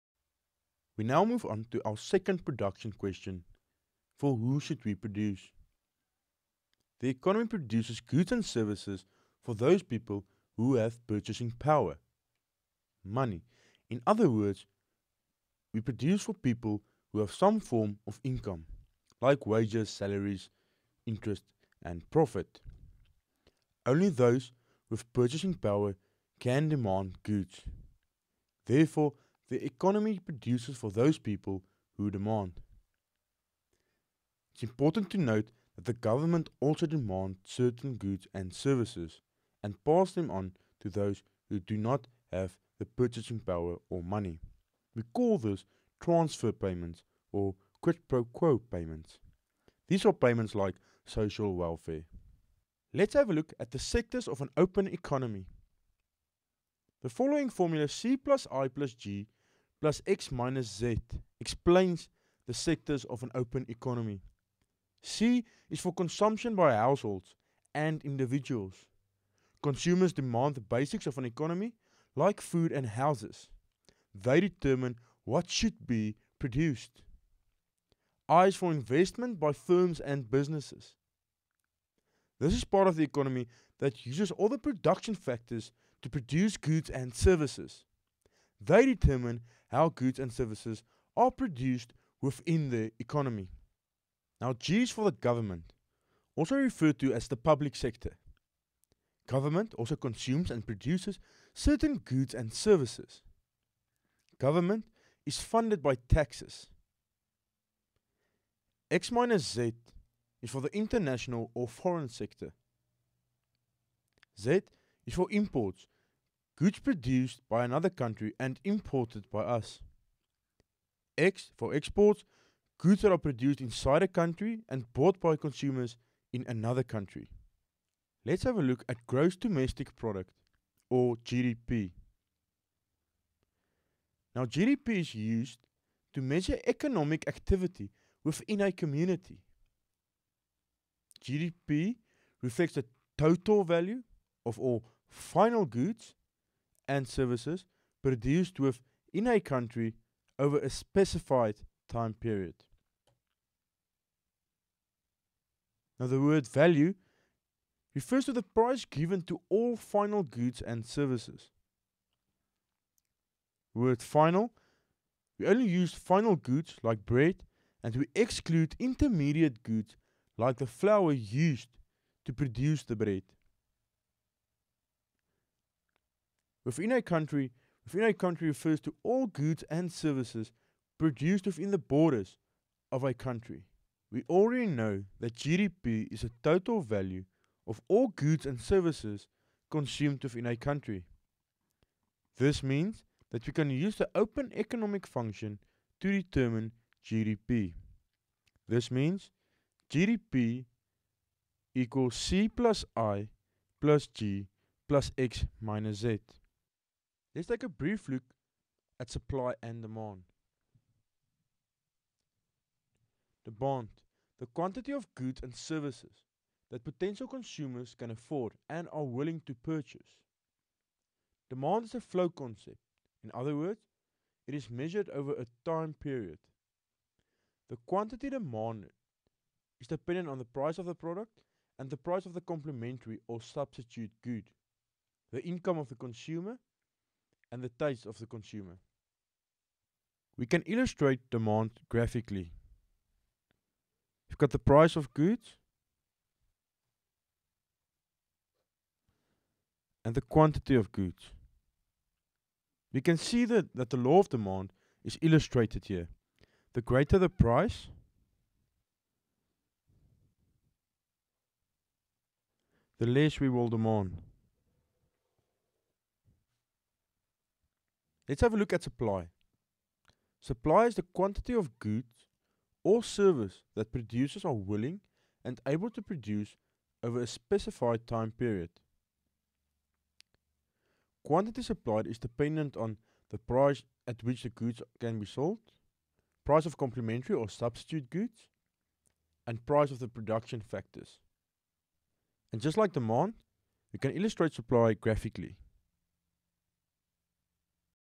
Economics for 1st year varsity introductory points
This video introduces the student to various concepts in first year economics at university. It deals with the various sectors in macroeconomics. Specifically in an open economy. this video considers the elements of gross domestic product or GDP.